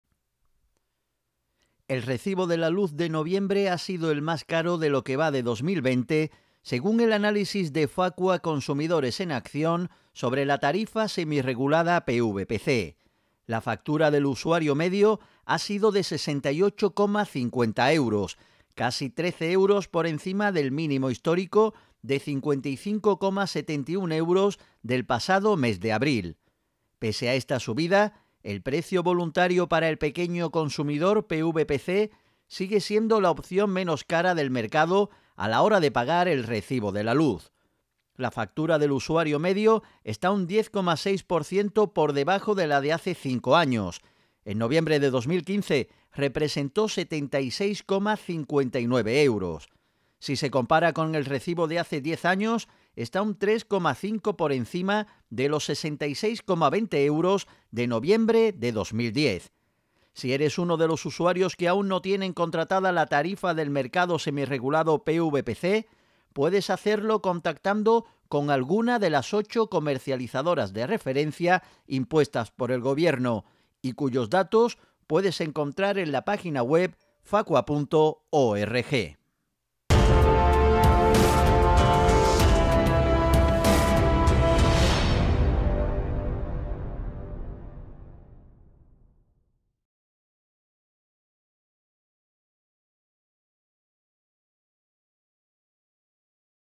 Este mes la luz ha subido un 6% para el usuario medio, que no te engañen: lo menos caro es el PVPC
El recibo de la luz de noviembre ha sido el más caro de lo que va de 2020, según el análisis de FACUA-Consumidores en Acción sobre la tarifa semirregulada PVPC. La factura del usuario medio ha sido de 68,50 euros, casi 13 euros por encima del mínimo histórico de 55,71 euros del pasado mes de abril.

Pese a esta subida, el Precio Voluntario para el Pequeño Consumidor (PVPC) sigue siendo la opción menos cara del mercado a la hora de pagar el recibo de la luz.

La factura del usuario medio está un 10,6% por debajo de la de hace cinco años: en noviembre de 2015 representó 76,59 euros. Si se compara con el recibo de hace diez años, está un 3,5% por encima de los 66,20 euros de noviembre de 2010.

Si eres uno de los usuarios que aún no tienen contratada la tarifa del mercado semirregulado PVPC, puedes hacerlo contactando con alguna de las 8 comercializadoras de referencia impuestas por el Gobierno y cuyos datos puedes encontrar en la página web FACUA.org.

El vídeo de FACUA forma parte de una campaña financiada por la Dirección General de Consumo del Ministerio de Consumo.

Más info en FACUA.org/16178.